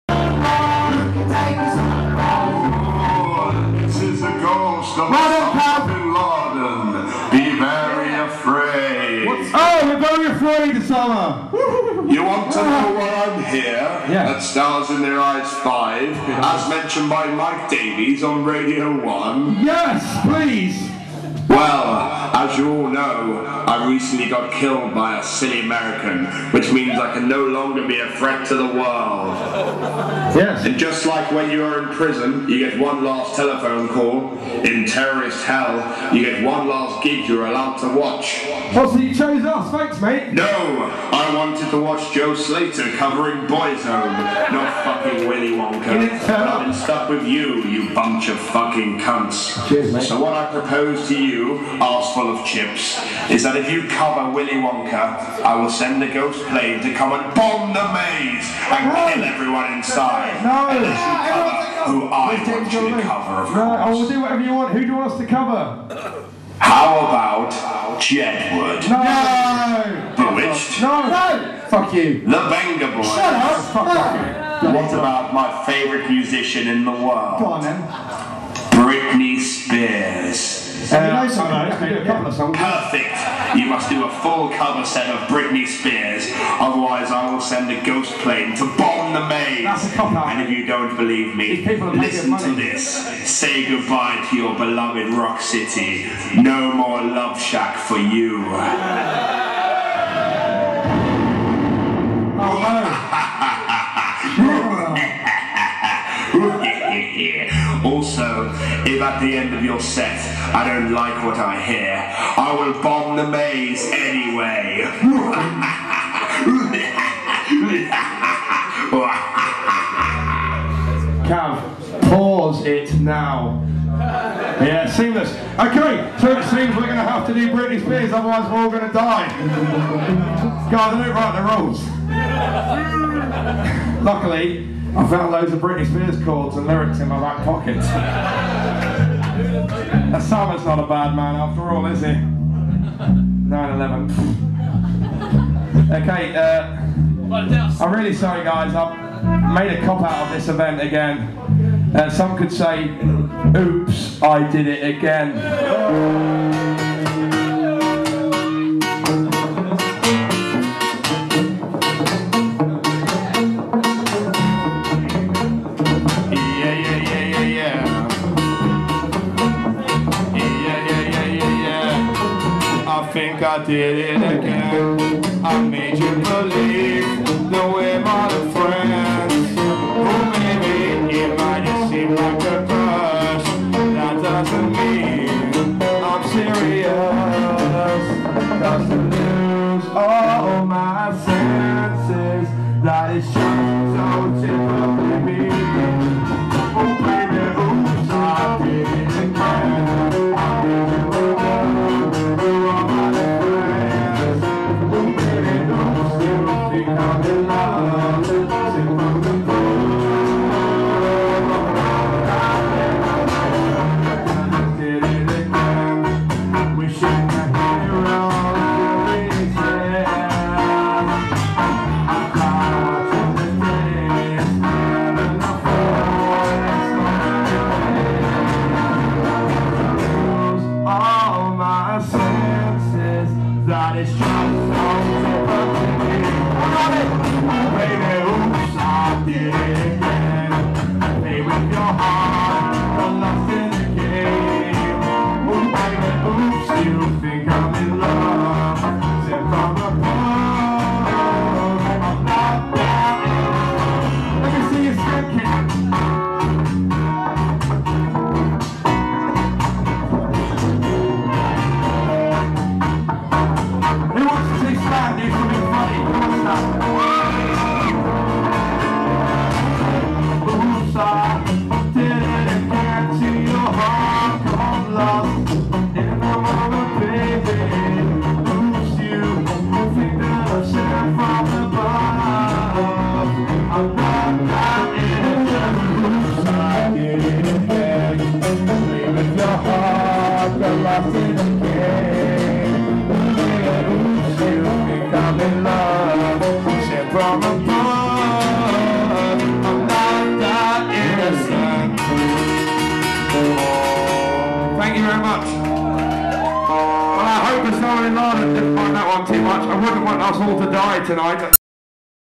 Arse full of Chips-Stars in Their Eyes 5 (00177)
...Arse full of Chips take on Willi Wonka and the Chocolate Factory live at The Maze,Nottingham.UK...22-05-11...